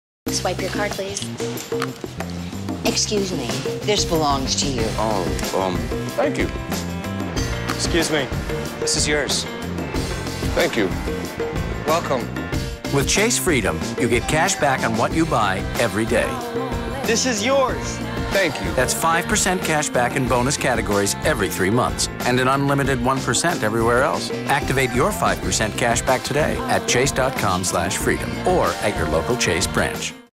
Petro TV - Chase Freedom
Chase Freedom campaign - works with Petro TV for hyper-local marketing